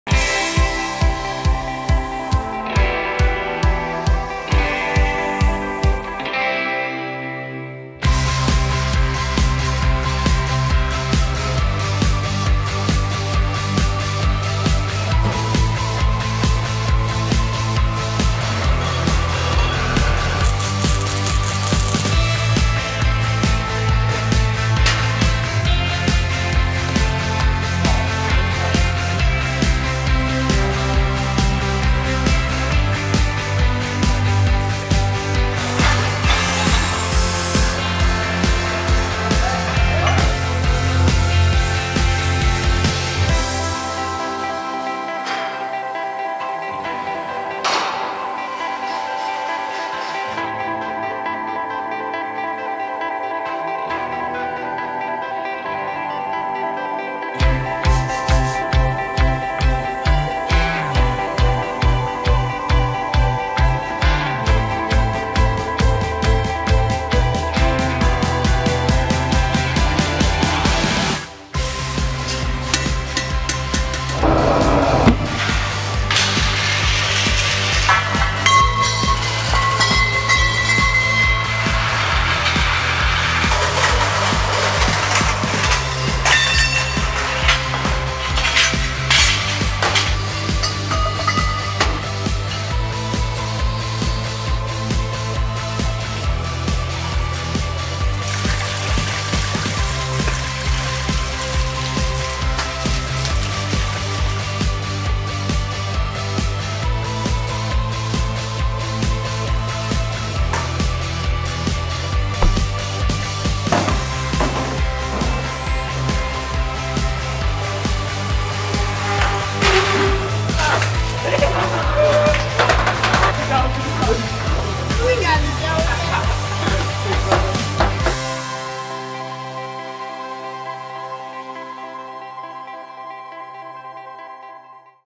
Helly Hansen Workwear Challenge 2015 – Team 4
This fall Helly Hansen’s Workwear Challenge 2015 took place in a series of High Schools in the wider Oslo area in Norway. Four groups of students entered in a competition to make the coolest Chain Reaction Machine, reflecting their majors. This team majors in Industrial technology (VG2) at Åssiden High School in Drammen.